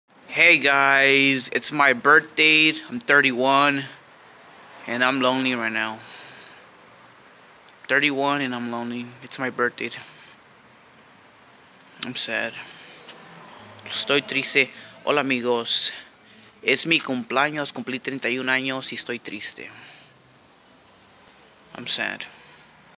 Its My 31st Birthday "Cumpli 31Años" 🎂 😭 im Alone "Estoy Solito"
Hey guys i'ts my Birthday es mi cumpleaños i Estoy triste!!..😭🎂